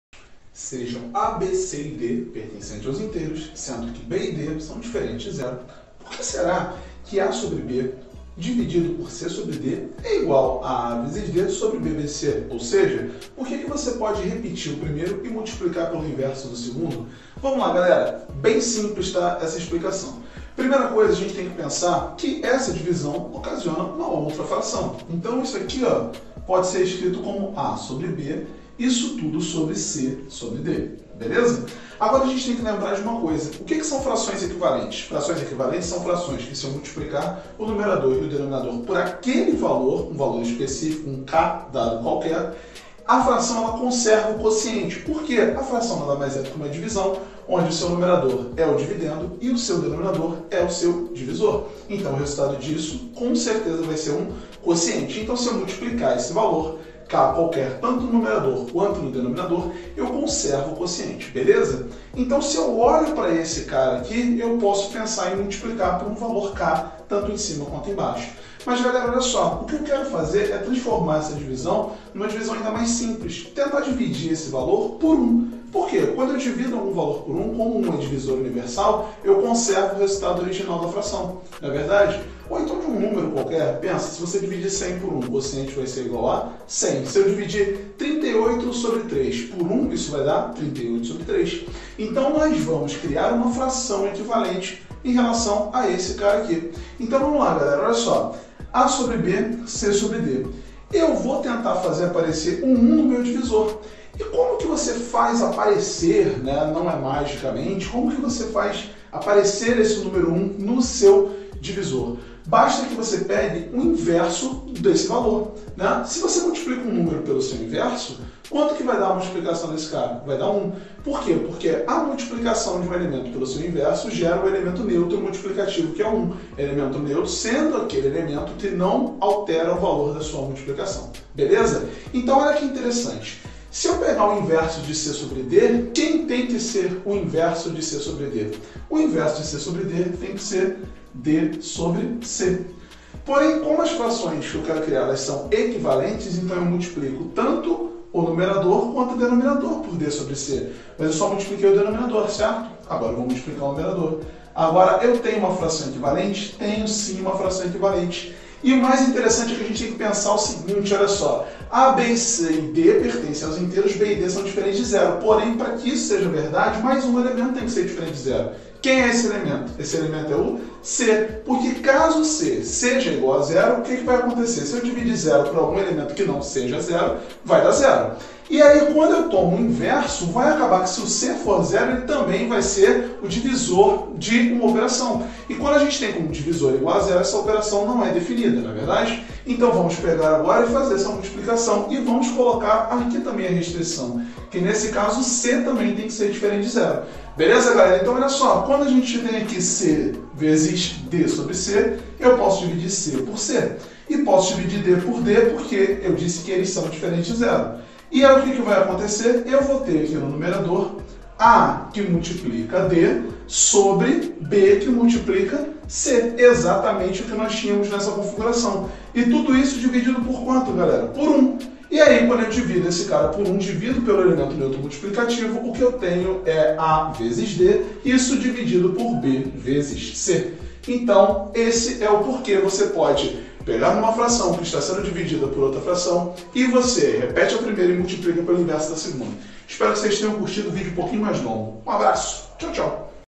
[VEJA !] DIVISÃO DE FRAÇÕES | REPETE A PRIMEIRA E MULTIPLICA PELO INVERSO DA SEGUNDA | DEMONSTRAÇÃO
SEMPRE TEVE ESSA DÚVIDA SOBRE A DIVISÃO ENTRE FRAÇÕES? ENTÃO VENHA VER AQUI NESTE VÍDEO INCRÍVEL E BEM DIDÁTICO.